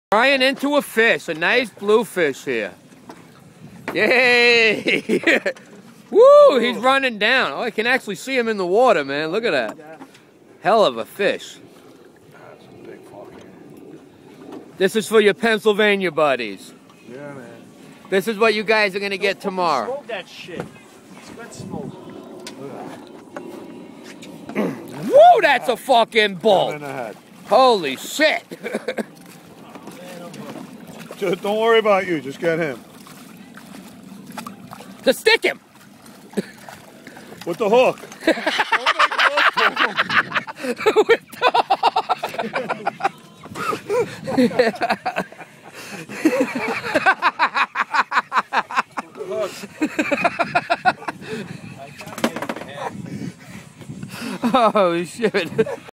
Bryan catches a bluefish on a outlaw custom fishing rod
Outlaw custom fishing rods